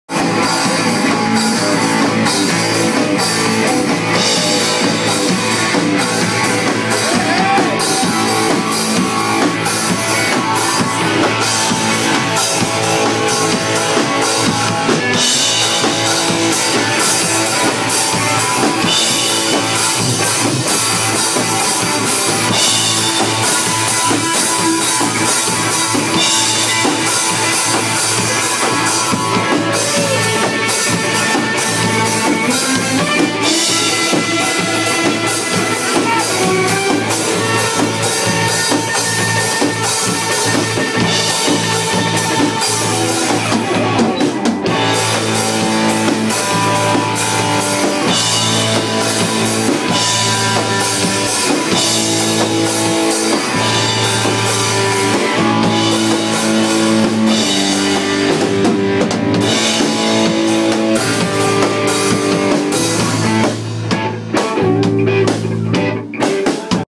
SMXLII Rock&Roll Final
Coqui, Gorila Amarillo y Vampiro (alias Mauricio Salazar) en gran Finale